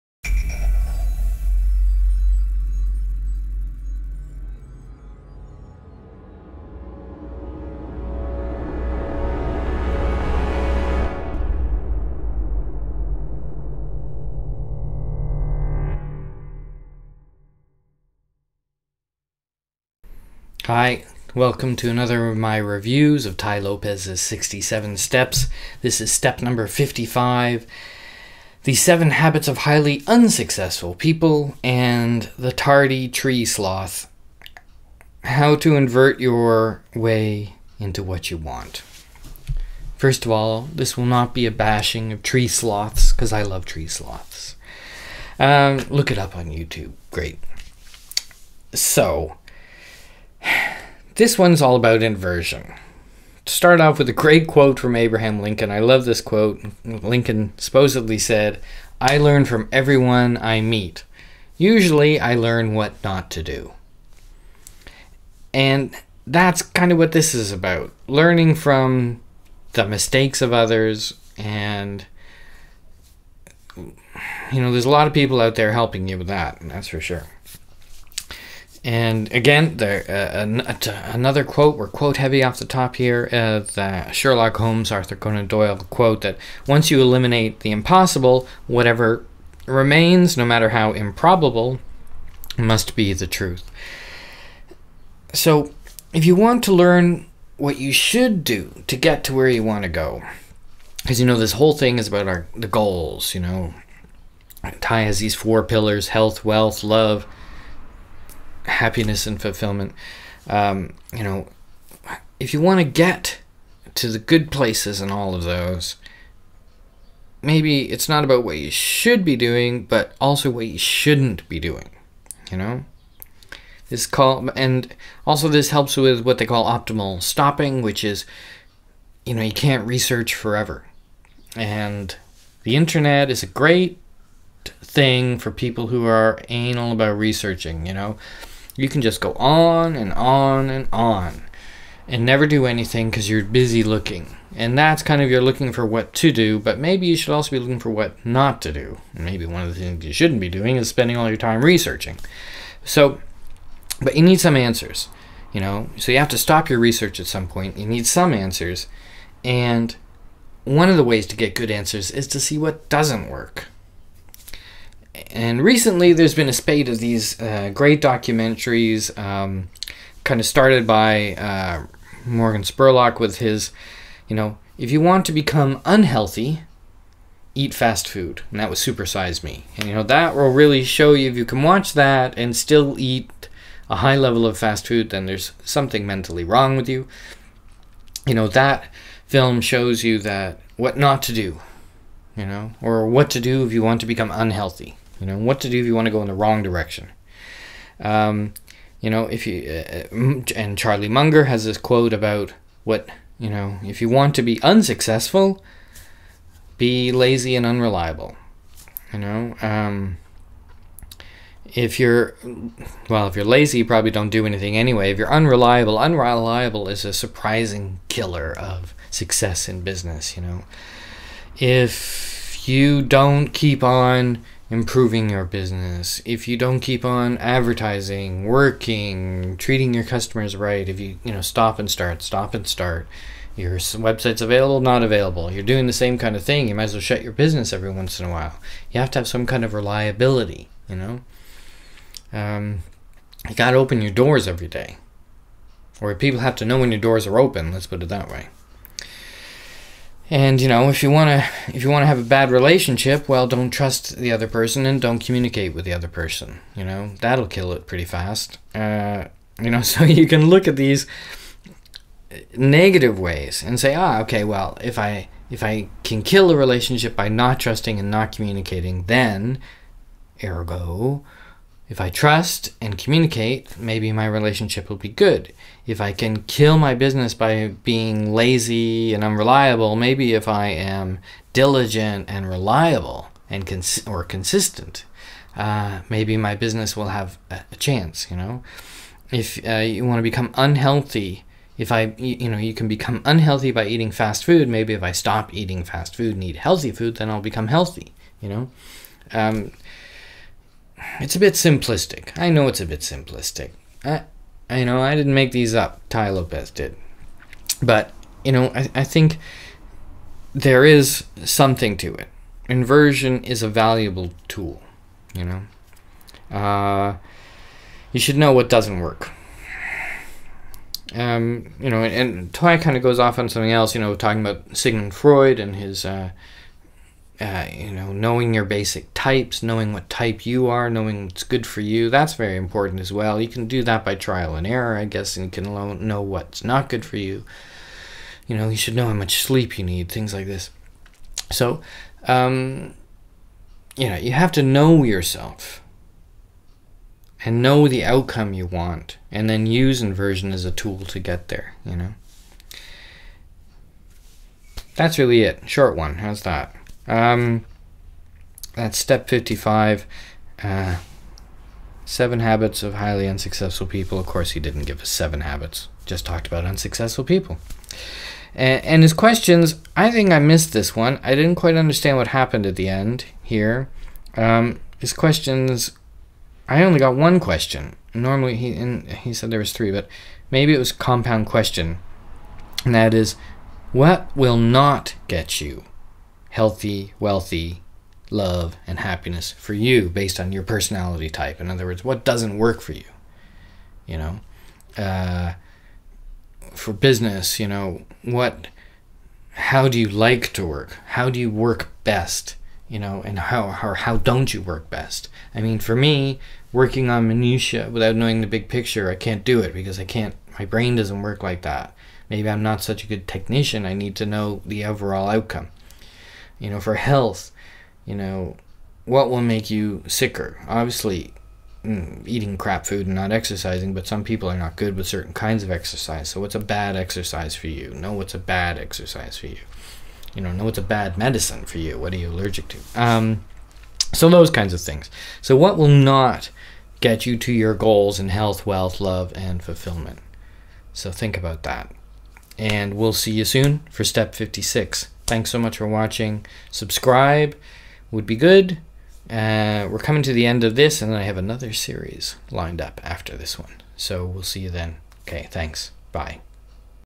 Tai Lopez 67 Steps Review - Step 55 - Inversion
My review of this step

55. The 7 Habits Of Highly Unsuccessful People & The Tardy Tree Sloth
How to invert your way into getting what you want.